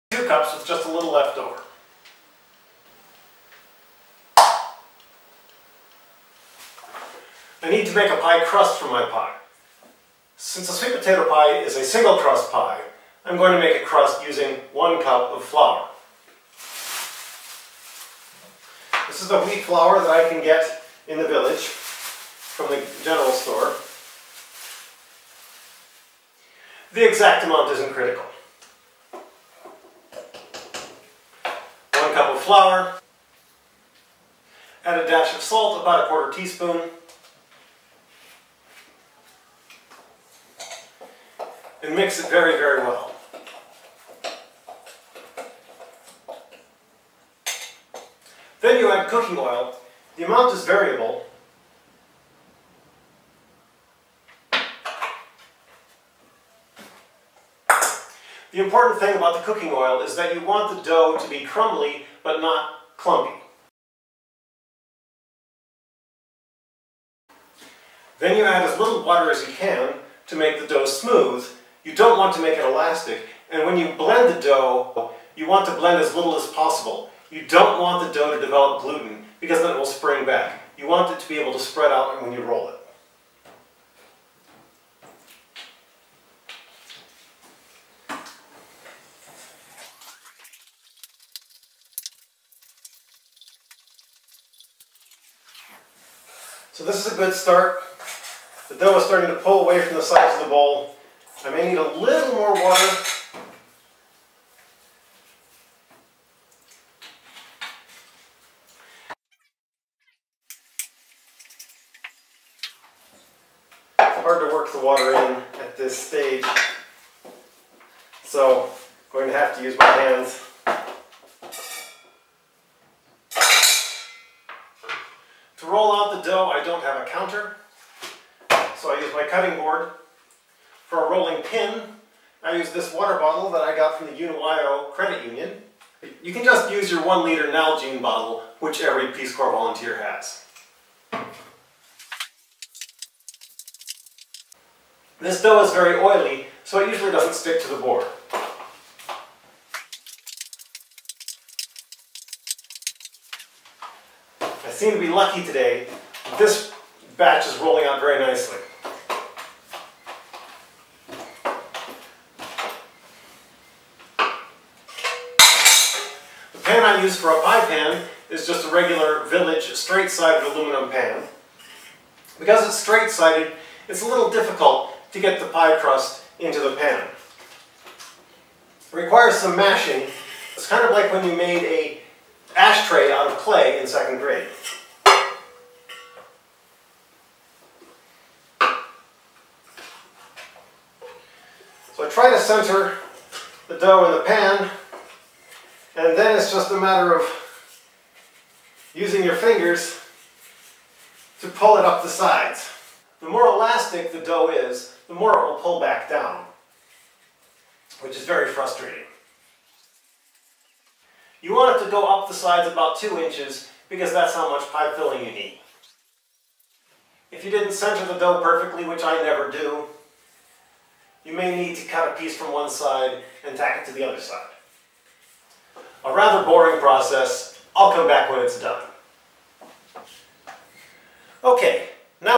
Sweet Potato Pie 2
How to make a sweet potato pie in a Peace Corps kitchen, part 2: pie crust.